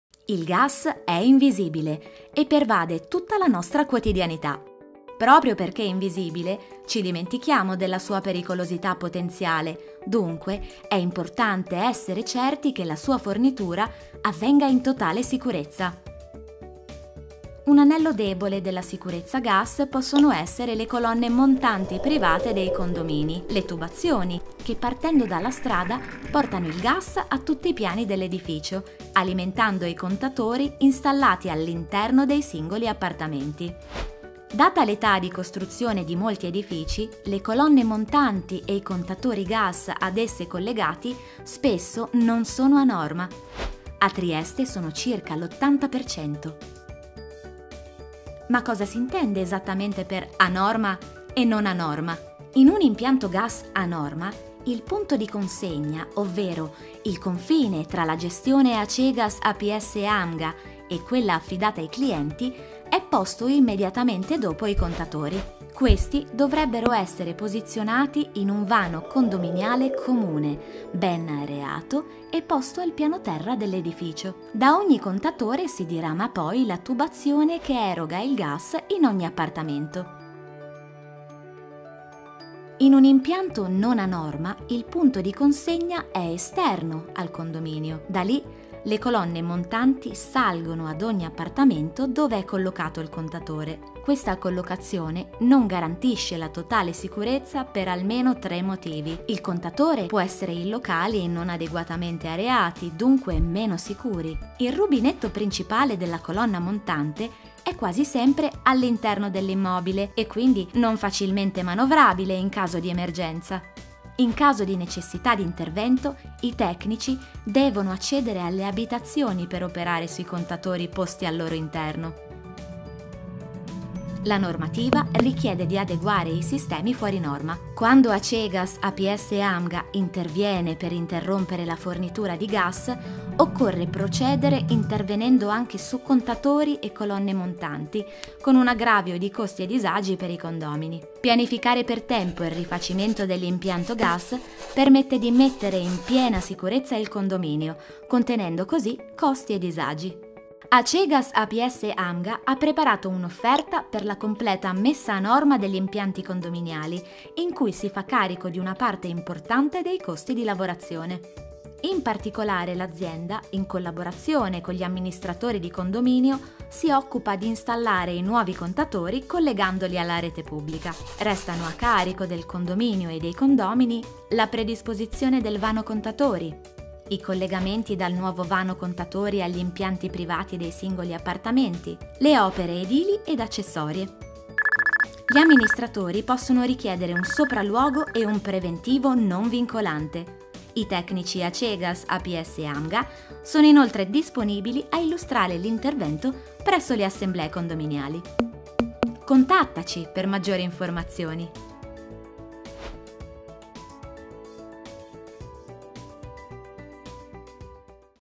Impianti gas condominiali: meglio metterli a norma
In Italia, oltre il 50% dei condomìni risalgono a metà del 1900: con il passare degli anni anche gli impianti interni del gas condominiali risultano anche meno sicuri rispetto a sistemi più moderni. La normativa si sta evolvendo in un’ottica di garanzia di maggiore sicurezza e continuità del servizio. In linea con le nuove leggi, AcegasApsAmga offre ad amministratori e condòmini un servizio per la messa a norma delle colonne montanti gas.
Insomma un check up completo per il proprio condominio finalizzato all’esigenza di adeguare gli impianti alle vigenti normative di sicurezza!